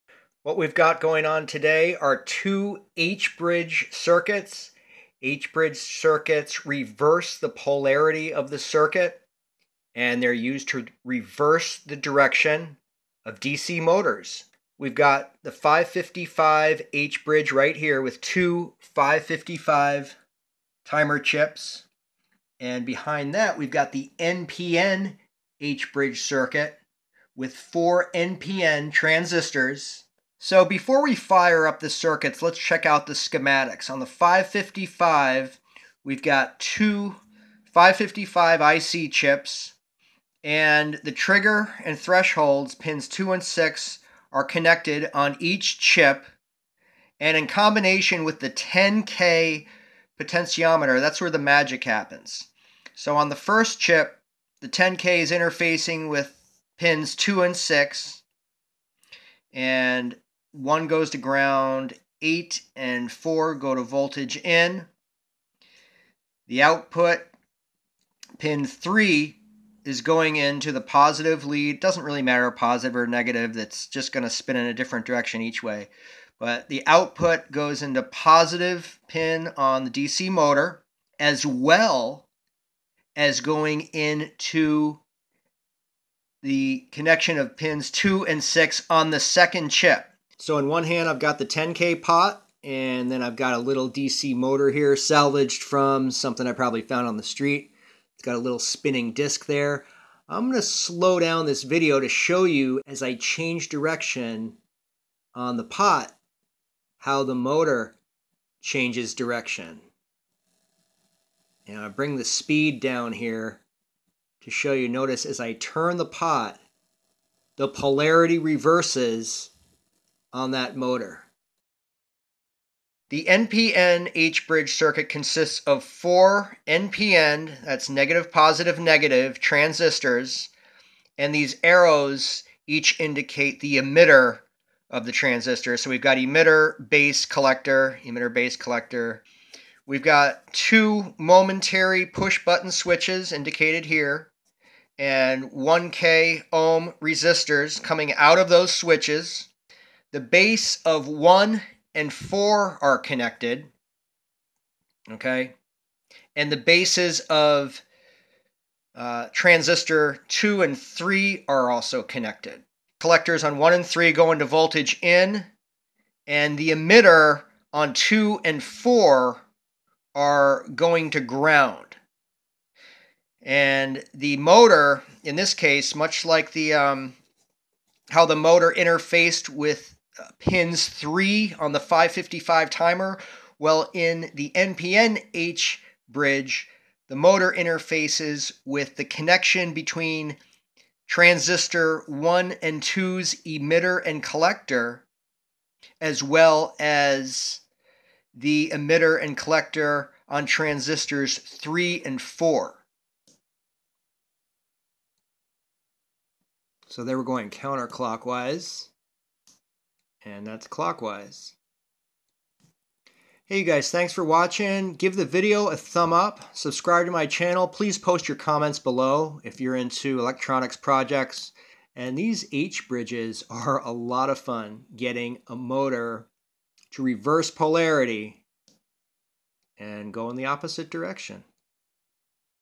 555 & NPN H-Bridge Forward/Reverse Motor Circuit
H-bridge circuits reverse polarity of DC motor, changing direction of motor rotation. NPN/PNP transistor combination is most common, but I didn't have PNPs on hand, so I'm using 4 NPNs in this video, which works fine. The 555 H-bridge is interesting in that a 10K potentiometer changes the polarity via pins 2 and 6 (trigger/threshold) on the ICs.